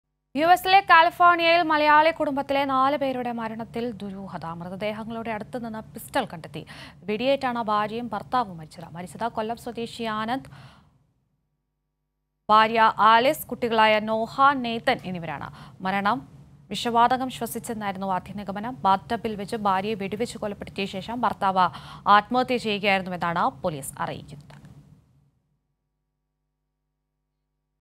വെടിയേറ്റ് മരിച്ചതോ, സ്വയം വെടിവെച്ച് മരിച്ചതോ? അമേരിക്കൻ മലയാളി കുടുംബത്തിന്റെ മരണത്തിൽ ദുരൂഹത
വെടിയേറ്റ് മരിച്ചതോ, സ്വയം വെടിവെച്ച് മരിച്ചതോ? മൃതദേഹങ്ങളുടെ അടുത്തുനിന്ന് പിസ്റ്റൾ കണ്ടെത്തിയതോടെ അമേരിക്കൻ മലയാളി കുടുംബത്തിന്റെ മരണത്തിൽ ദുരൂഹത
Mystery in the death of the American Malayali family at california